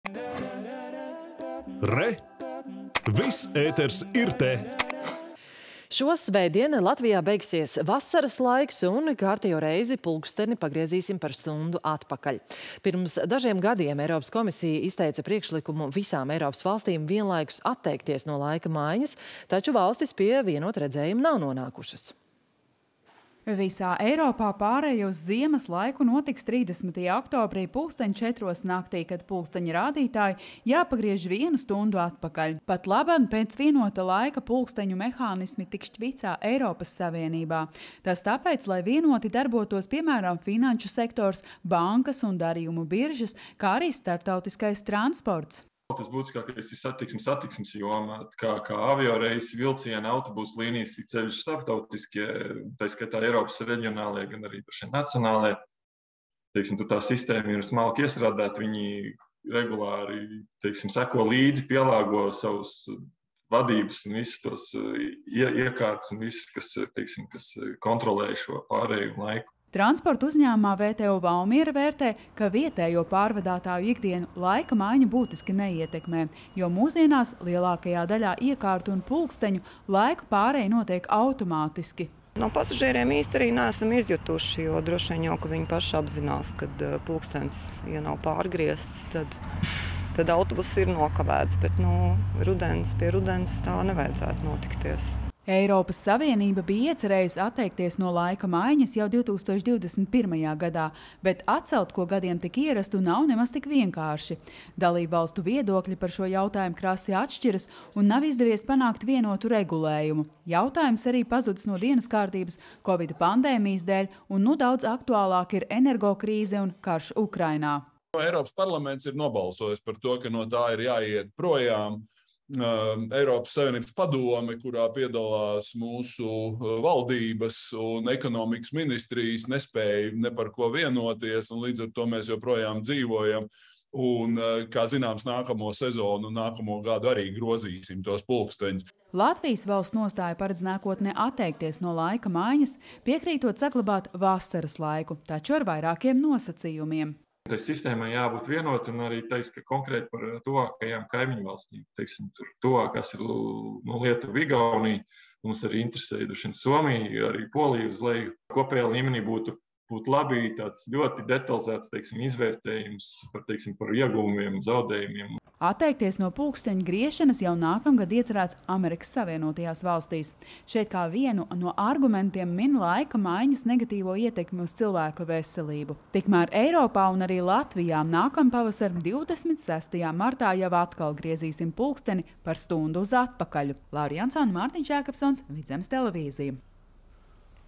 Atteikšanās no laika maiņas pagaidām nenotiks
Šo svētdien Latvijā beigsies vasaras laiks un kārtējo reizi pulksteni pagriezīsim par stundu atpakaļ. Pirms dažiem gadiem Eiropas Komisija izteica priekšlikumu visām Eiropas valstīm vienlaikus atteikties no laika maiņas, taču valstis pie vienota redzējuma nav nonākušas.